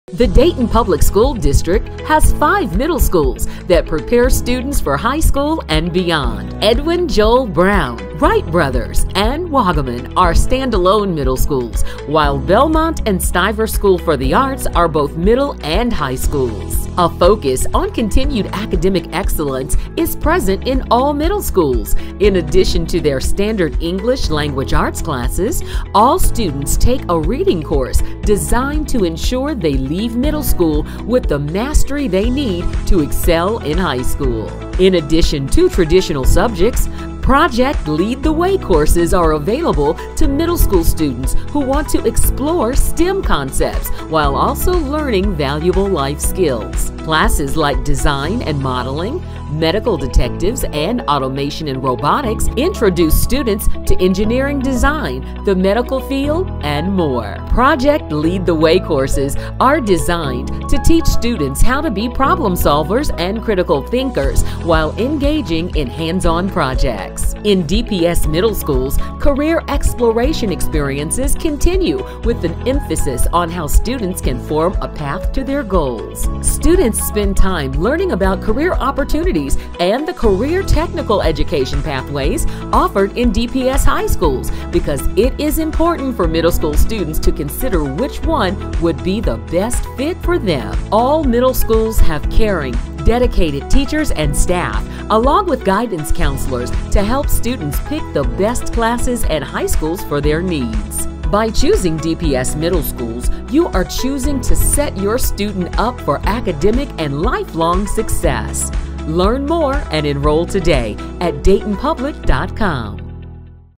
Choose DPS Middle Schools
The Dayton Public School District has 5 middle schools that prepare students for high school and beyond.

Edwin Joel Brown, Wright Brothers, and Wogaman are stand-alone middle schools, while Belmont and Stivers School for the Arts are both middle and high schools.